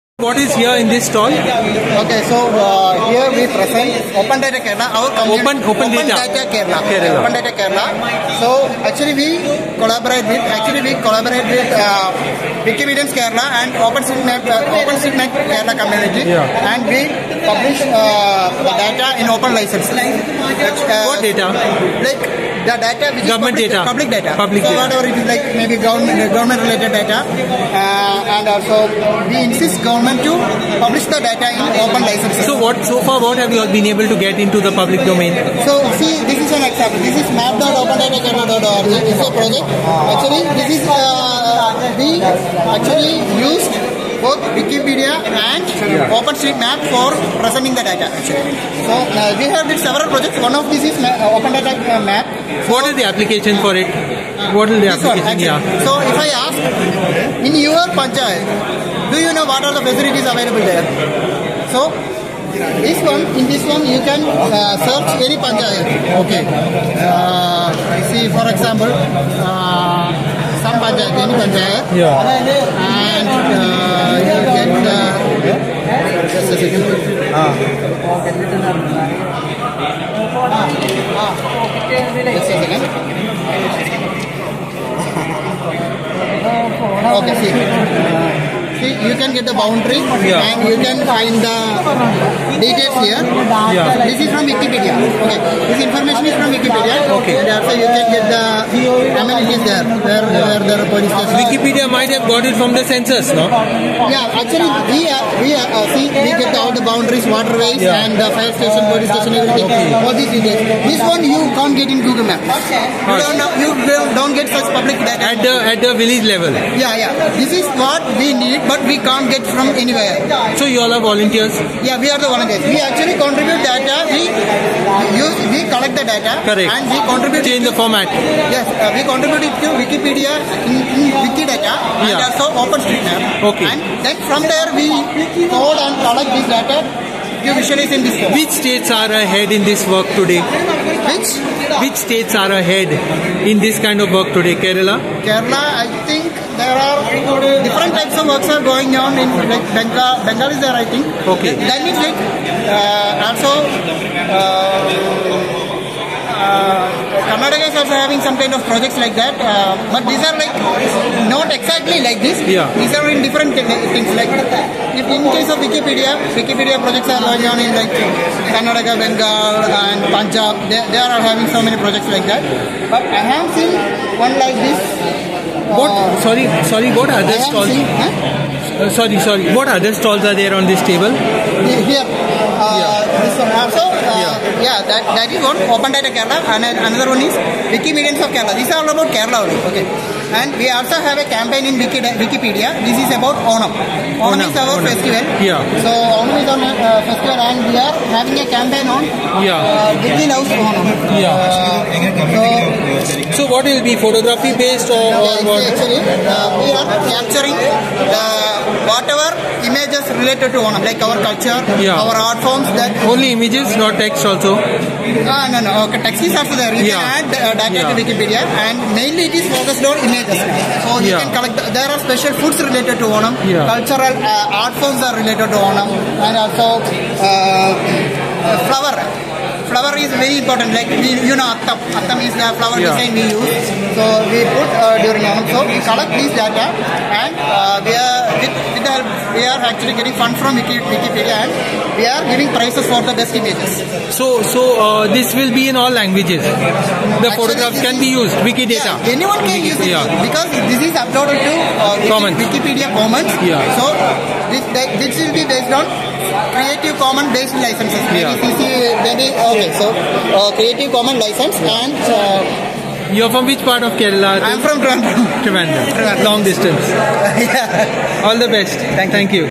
OpenData Kerala... what's it all about
Publishing publicly-available data. Recorded by FN.